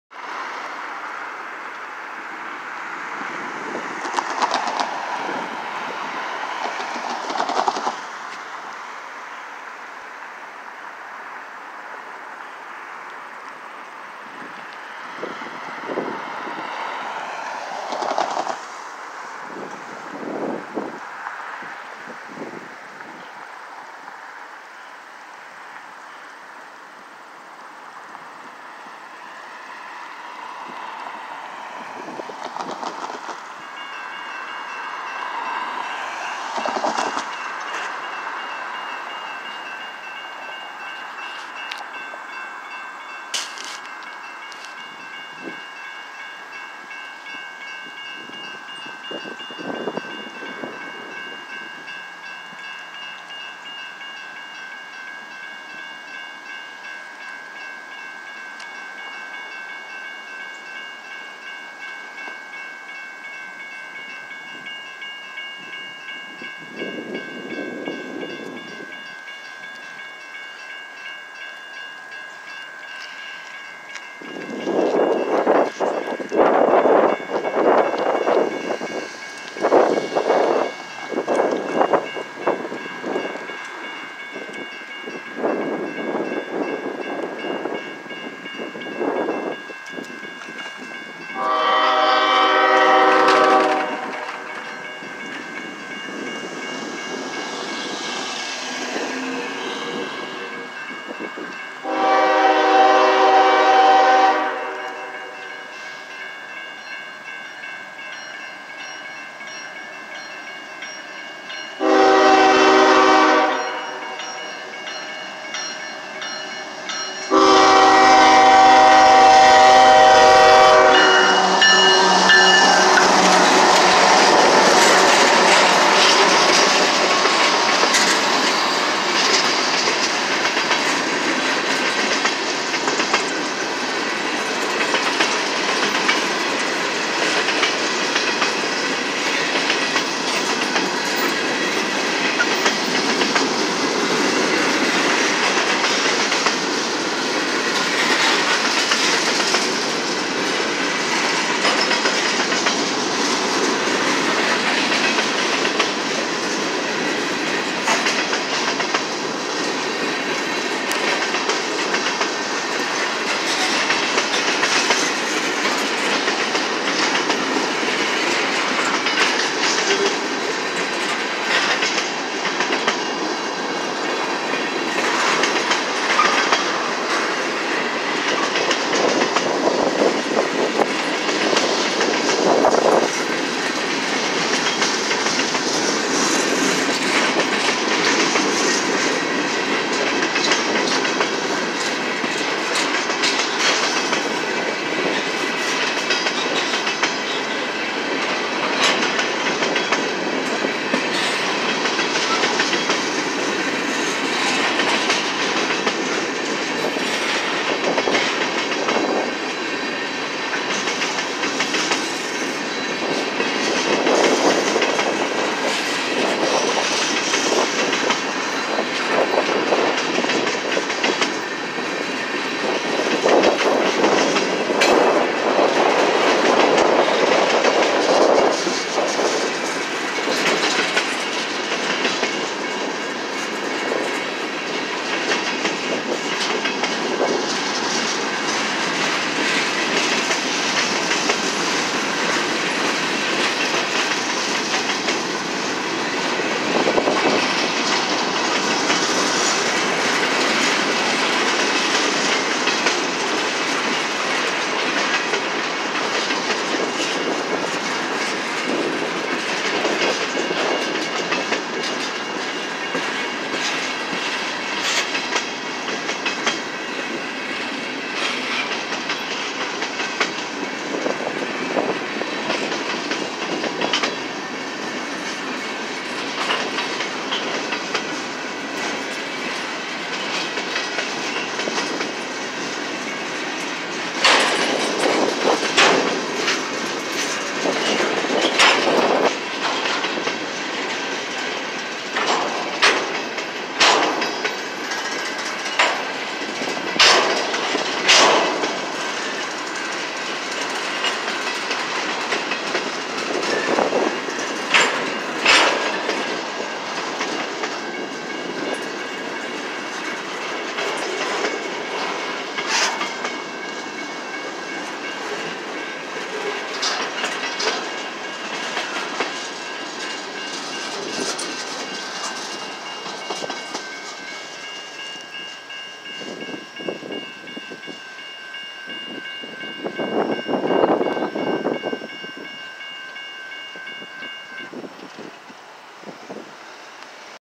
CSX freight train (10-12-2019)
CSX 715 ES44AC-H, CSX 3040 ES44AC-H heading to the yard as it lead the eastbound.